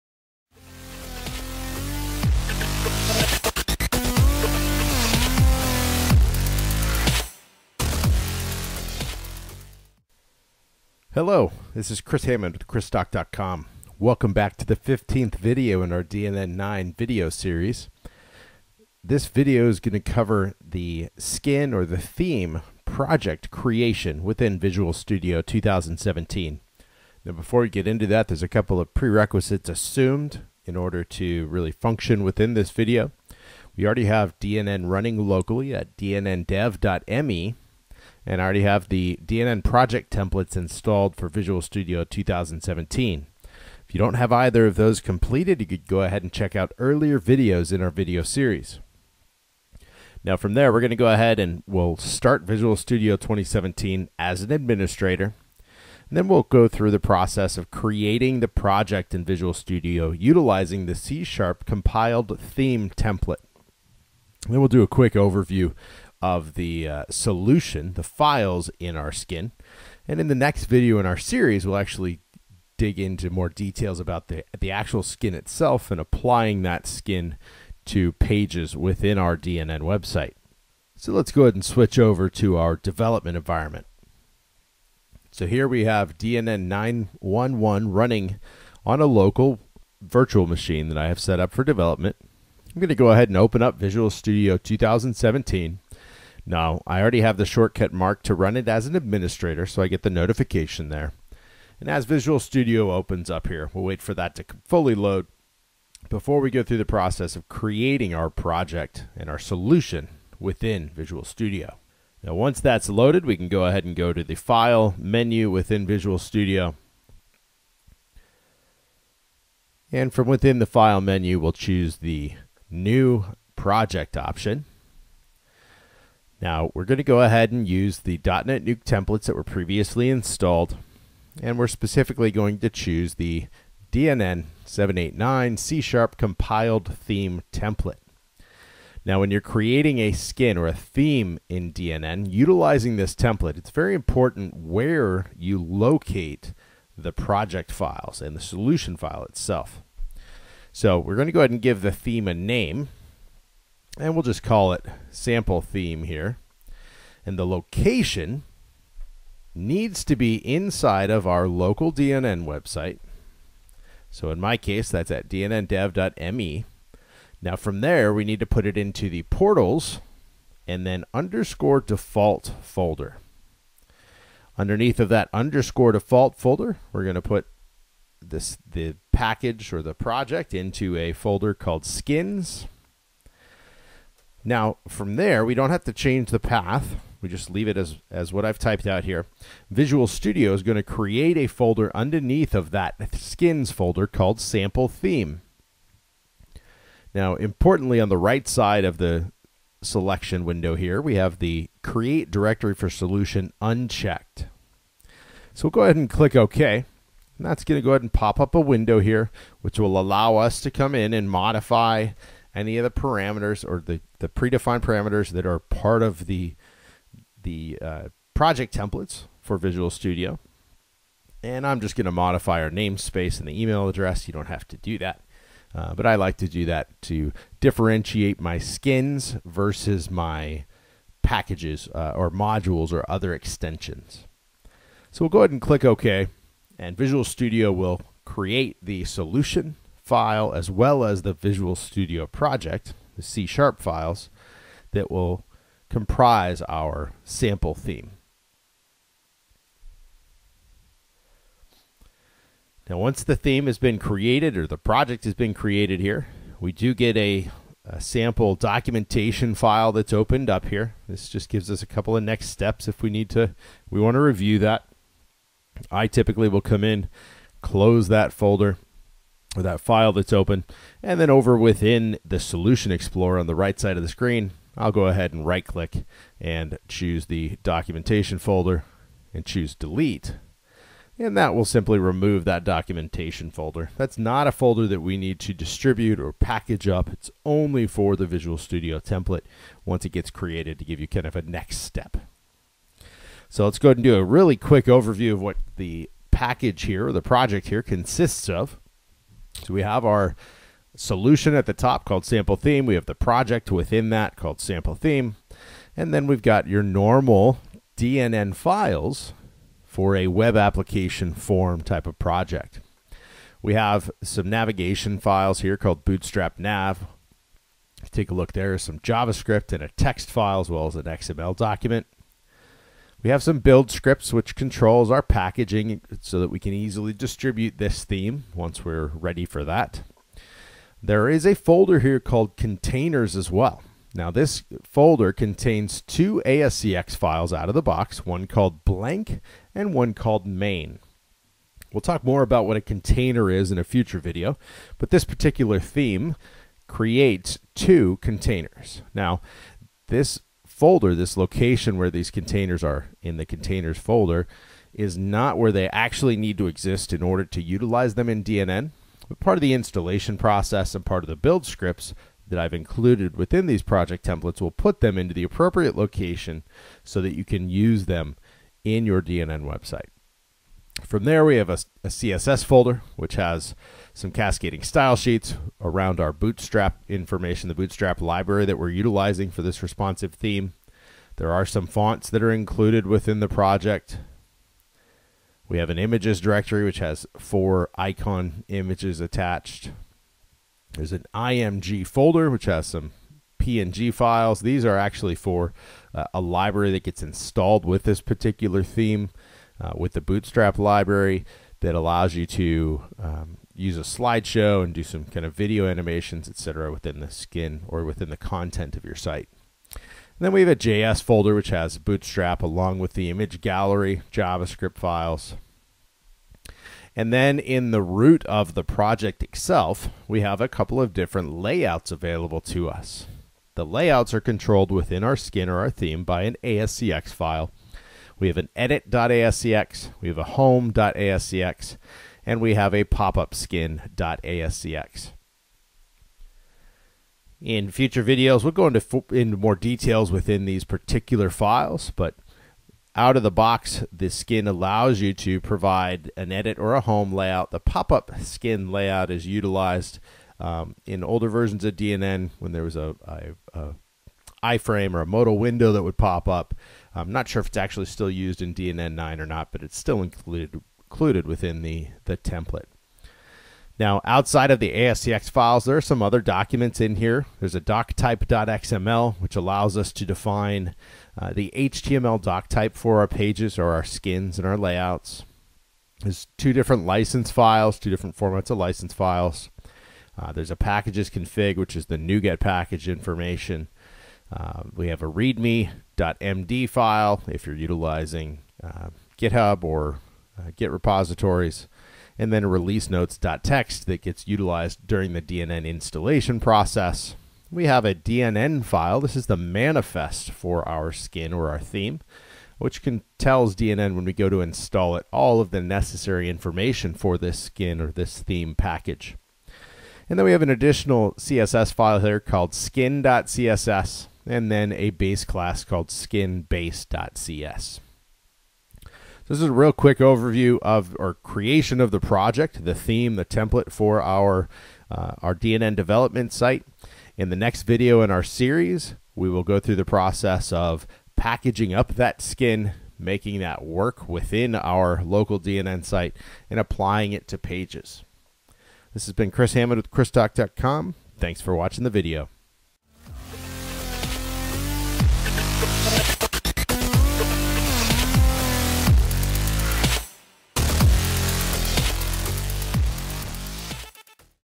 DNN 9 Series Video 15 - Skin/Theme project creation in Visual Studio 2017
DNN 9 Video Series - Skin/Theme Project Creation

Prerequisites
    Running DNNDEV.ME (locally)
    DNN project templates installed

Load Visual Studio 2017 as an Administrator

Create Project using C# Compiled Theme template

Solution Overview

Next Video : Skin/Theme Project Overview